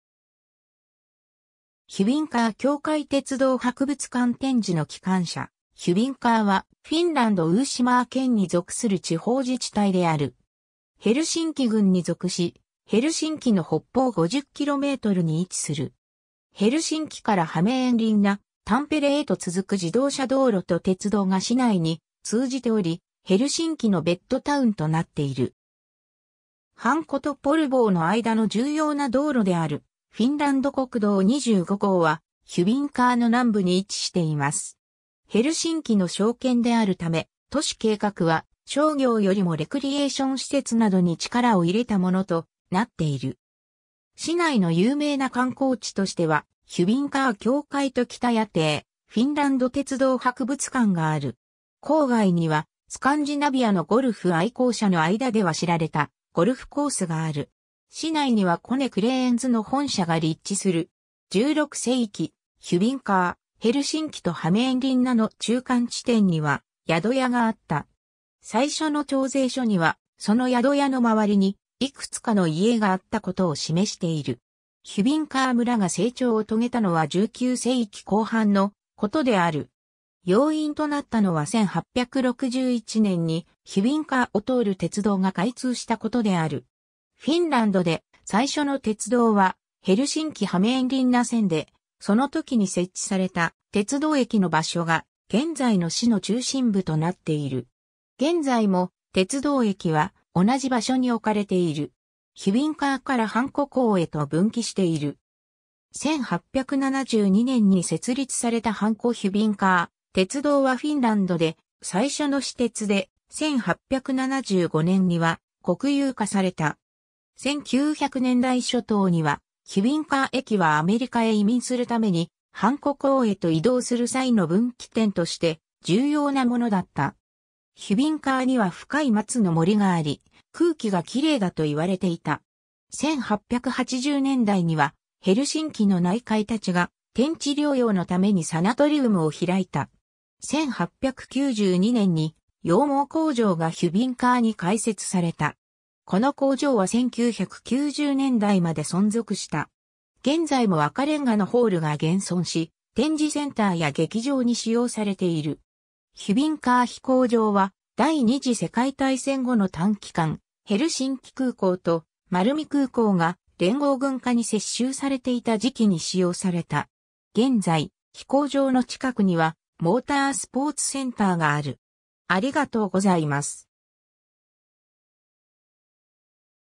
ヒュヴィンカー
ヒュヴィンカー教会 鉄道博物館展示の機関車 ヒュヴィンカー(フィンランド語: Hyvinkää, スウェーデン語: Hyvinge)は、フィンランド・ウーシマー県に属する地方自治体である。
ヘルシンキ郡に属し、ヘルシンキの北方50kmに位置する。
 ヘルシンキからハメーンリンナ、タンペレへと続く自動車道路(E12号線)と鉄道(VR)が市内に通じており、ヘルシンキのベッドタウンとなっている。
ハンコとポルヴォーの間の重要な道路であるフィンランド国道25号は、ヒュヴィンカーの南部に位置しています。
ヘルシンキの商圏であるため、都市計画は商業よりもレクリエーション施設などに力を入れたものとなっている。
 市内の有名な観光地としてはヒュヴィンカー教会とキタヤ邸、フィンランド鉄道博物館がある。
郊外にはスカンジナビアのゴルフ愛好者の間では知られたゴルフコースがある。
 市内にはコネ・クレーンズ（コネのクレーン部門が前身）の本社が立地する。
 16世紀、ヒュヴィンカー、ヘルシンキとハメーンリンナの中間地点には宿屋があった。
最初の徴税書には、その宿屋のまわりにいくつかの家があったことを示している。
ヒュヴィンカー村が成長を遂げたのは19世紀後半のことである。
要因となったのは1861年に ヒュヴィンカーを通る鉄道が開通した事である。
フィンランドで最初の鉄道はヘルシンキ・ハメーンリンナ線で、そのときに設置された鉄道駅の場所が現在の市の中心部となっている。
現在も鉄道駅は同じ場所に置かれている。
ヒュヴィンカーからハンコ港へと分岐している。
1872年に設立されたハンコ・ヒュヴィンカー鉄道はフィンランドで最初の私鉄で、1875年には国有化された。
1900年代初頭にはヒュヴィンカー駅はアメリカへ移民するためにハンコ港へと移動する際の分岐点として重要なものだった。
 ヒュヴィンカーには深い松の森があり、空気が綺麗だと言われていた。
1880年代にはヘルシンキの内科医たちが転地療養のためにサナトリウムを開いた。
 1892年に羊毛工場がヒュヴィンカーに開設された。
この工場は1990年代まで存続した。
現在も赤煉瓦のホールが現存し、展示センターや劇場に使用されている。
ヒュヴィンカー飛行場は第二次世界大戦後の短期間、ヘルシンキ空港とマルミ空港が連合軍下に接収されていた時期に使用された。
現在、飛行場の近くにはモータースポーツセンターがある。